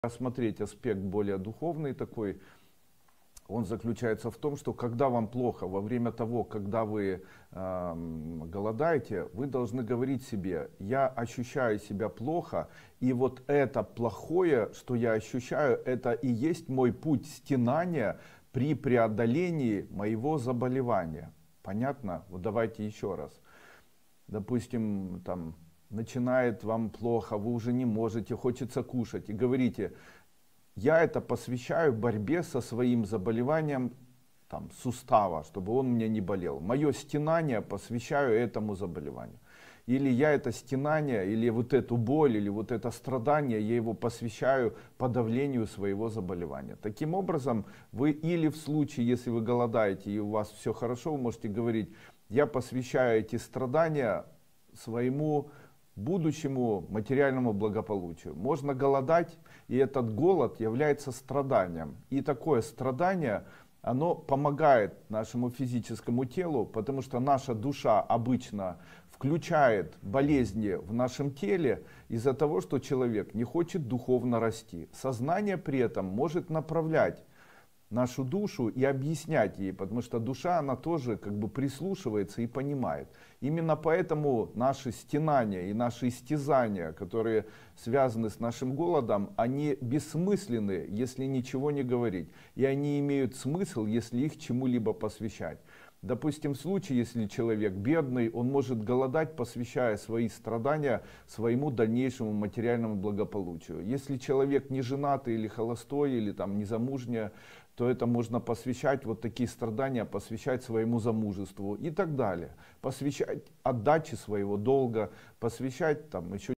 Стенания во время добровольного голодания @Андрей Дуйко
Практика добровольного голодания. Голод как перезагрузка организма. Стенания во время добровольного голодания.
- Обязательно очистительное голодание нужно чему-то посвящать. Духовная поддержка себя во время лечебного голодание:
 – необходимо, когда вам особенно трудно терпеть,  поддерживать себя словами: - «Я посвящаю это голодание, мои стенания ,  избавлению от моего заболевания…» или «Я посвящаю это голодание, моему будущему материальному благополучию».
Голод вызывает страдание и поскольку наша душа понимает, что человек не хочет духовно расти, то мы можем воспитывать свое тело через голод, объясняя душе, что мы эти страдания посвящаем своему духовному росту.